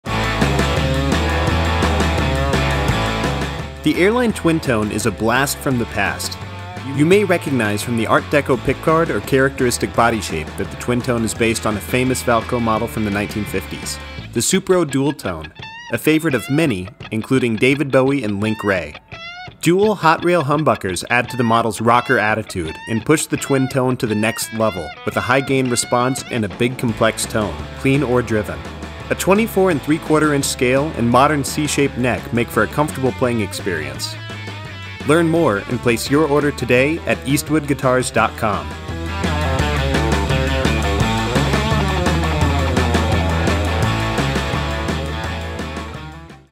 Showcase: Airline Twin Tone
The next step in the evolution of the Supro Dual Tone from the 50's!
Basswood body, bolt on Maple neck with Rosewood fingerboard, 24 3/4" scale length, 3 way switching, two volume and two tone knobs, and two hot rail humbuckers.